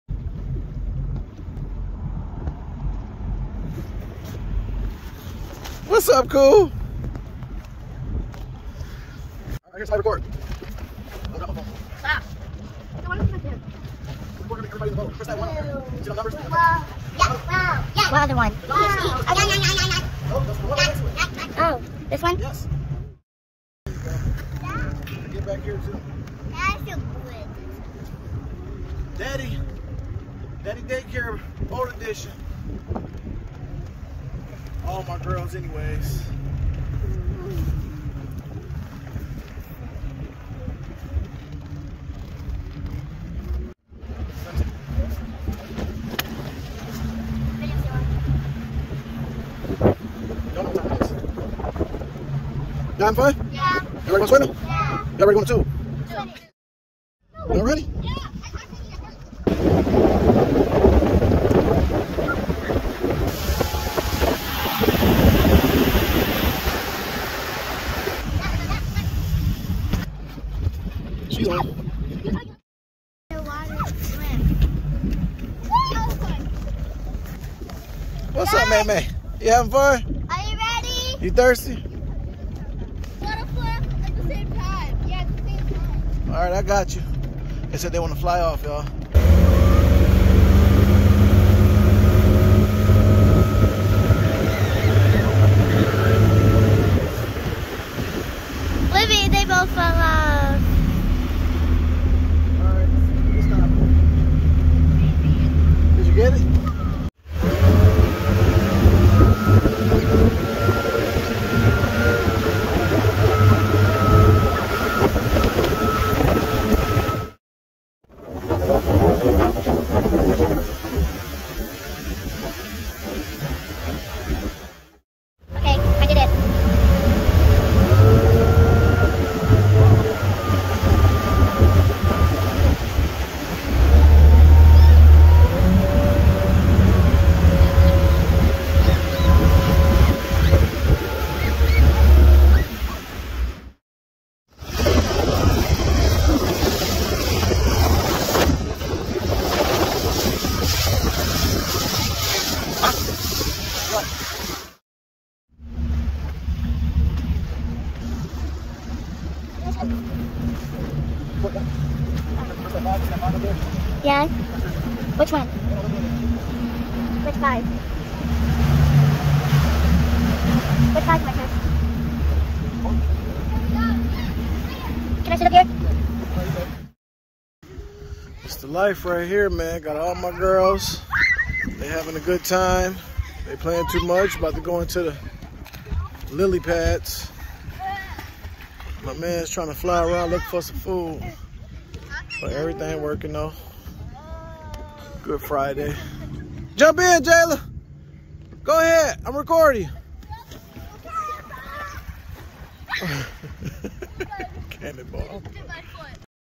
dads life with all girls be like
when your daughters want to go out on the boat and tubing